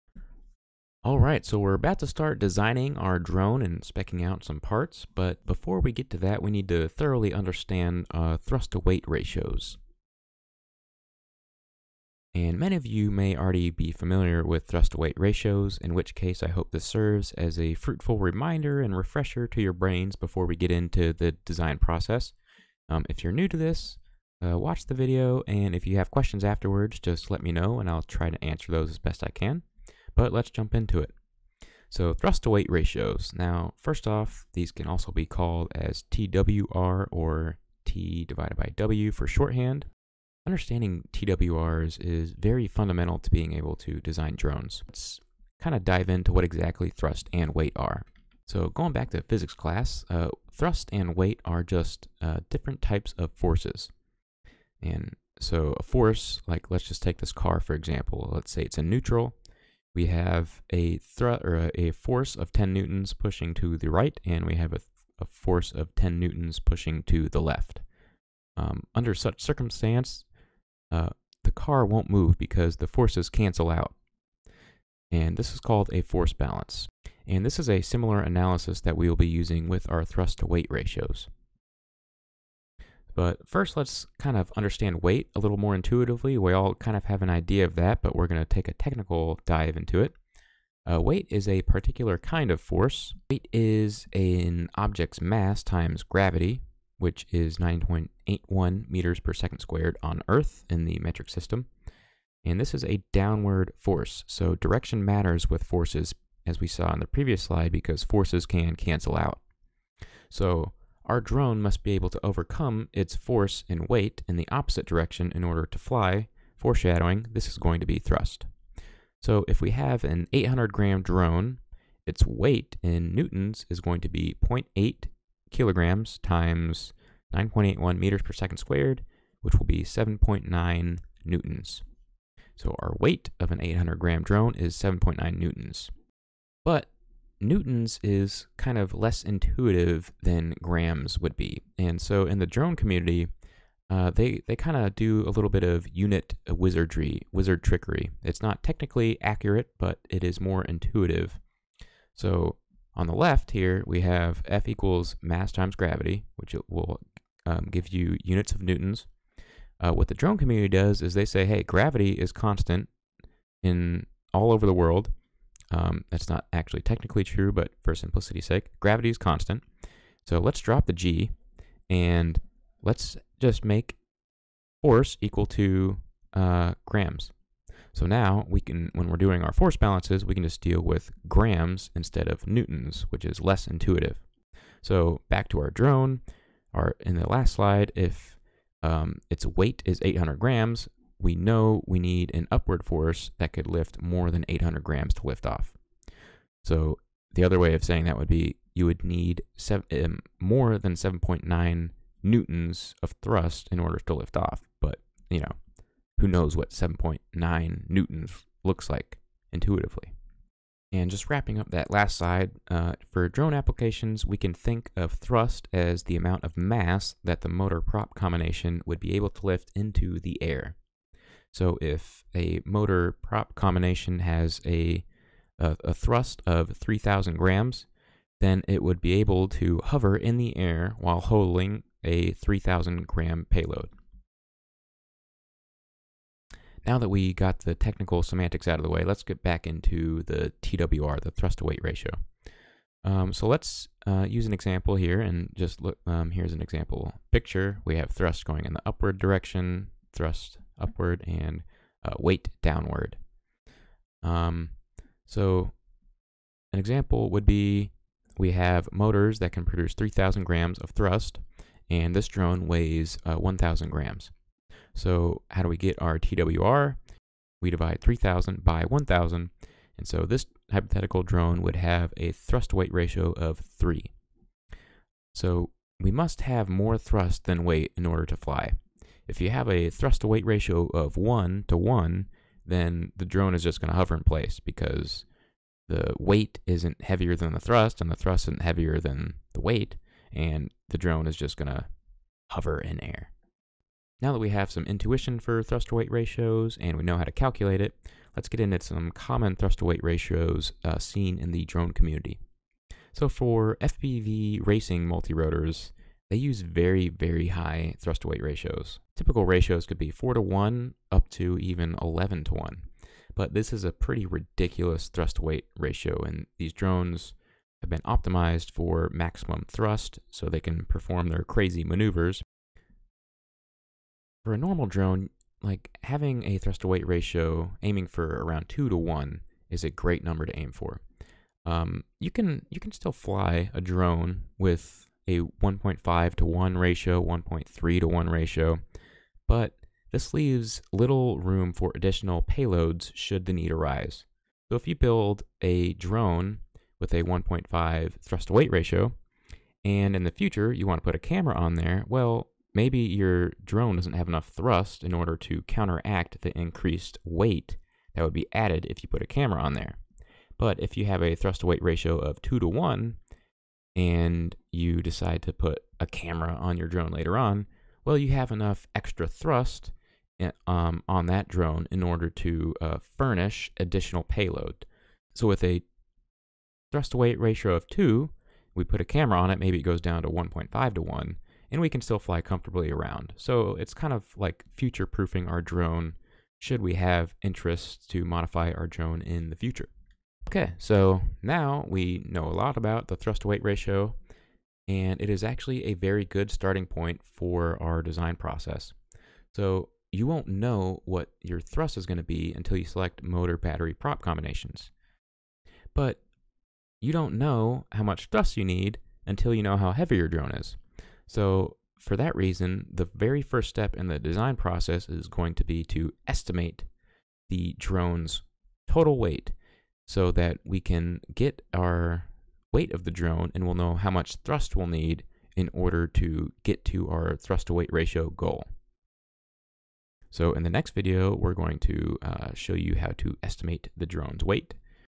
Thrust to Weight Ratios | A Drone Design Foundation
Learn what the heck thrust to weight ratios are (TWR or TW), and why they are important to drone design.

In the drone designing process, the first step should be determining a target thrust to weight (TW) ratio that is desirable for the type of drone you want to build.

You will want different TW ratios for different types of drones.

If you'd like a full 6 hour curriculum to jumpstart your drone engineering learning path, check out this course!